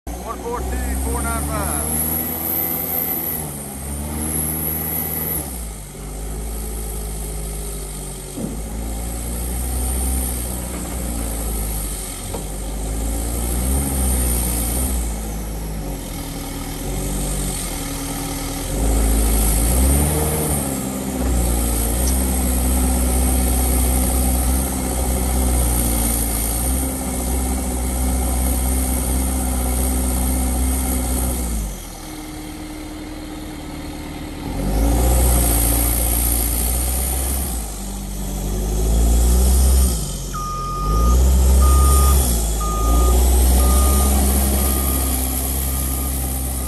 GRADALL 534D-10-45 For Sale
OROPS, 2 SECTION BOOM W/ FORKS, 4X4X4, 5000# CAP (SHOWS 3349 HRS) S/N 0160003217

 [GRADALL 534D10-45 TELESCOPIC FORKLIFT]